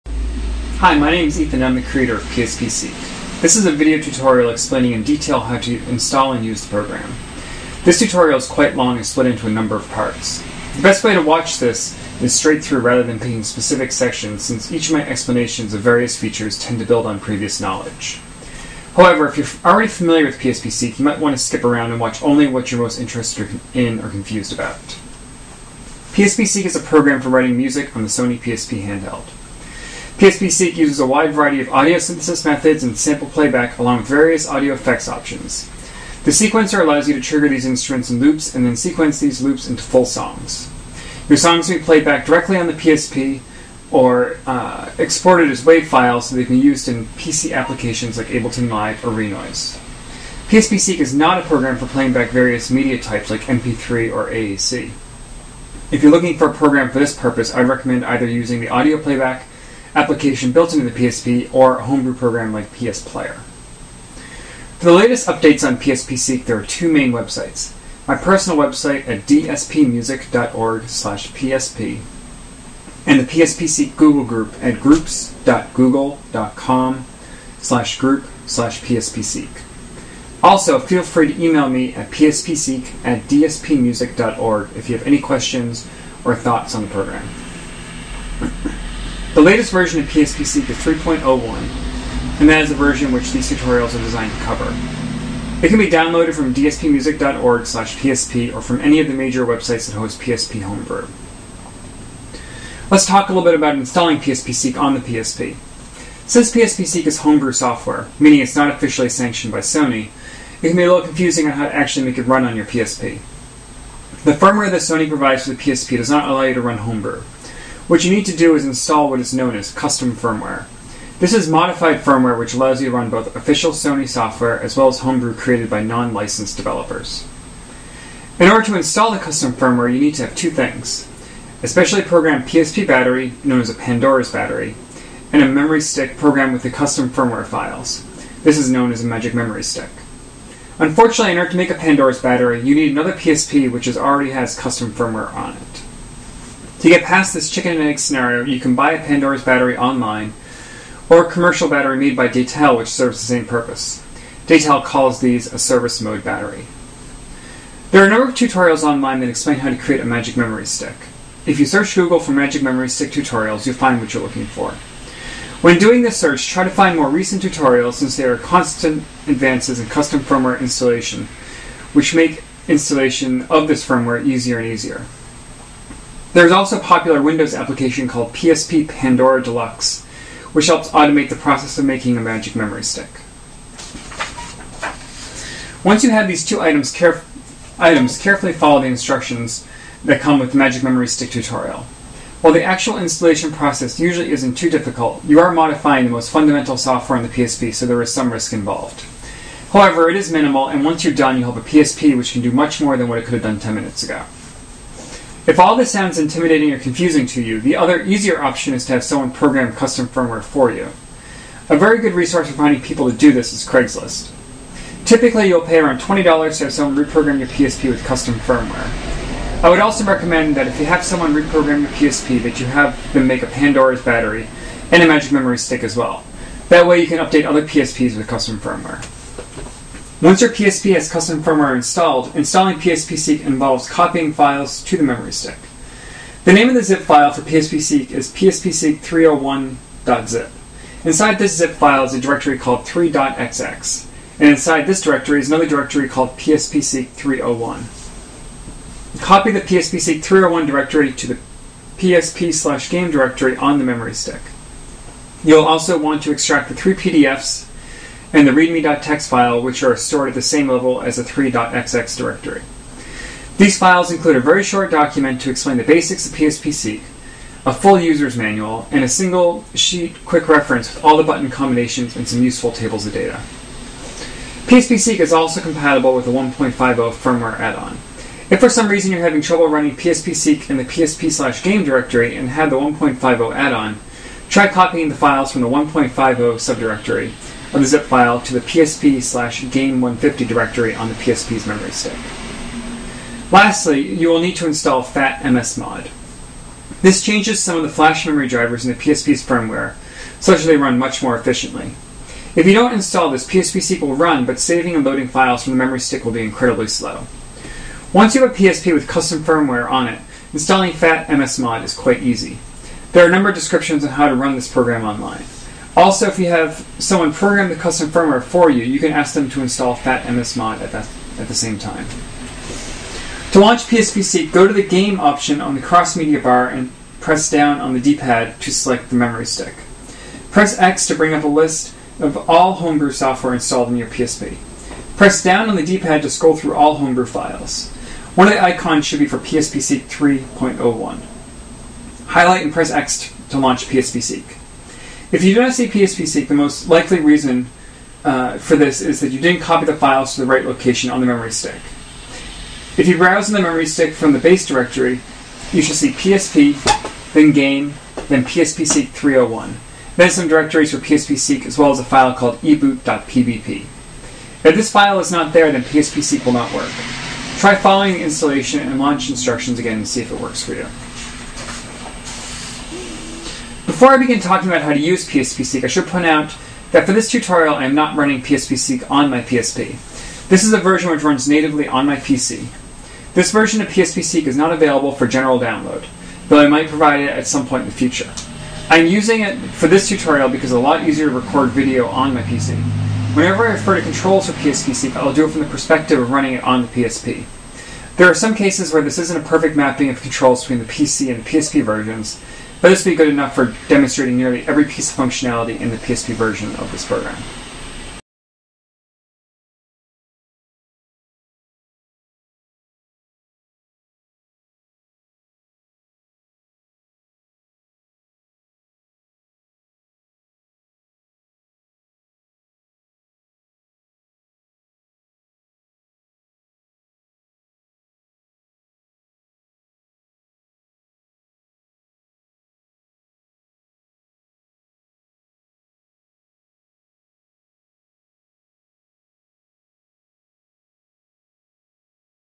PSPSeq 3.01 tutorial part 1 - introduction and installation
this is part 1 of a tutorial on how to make music with PSPSeq.  this part of the tutorial covers the very basic concepts as well as how to install custom firmware on your PSP so you can run PSPSeq.  if you already have PSPSeq installed or are familiar with homebrew, there's no need to watch this tutorial.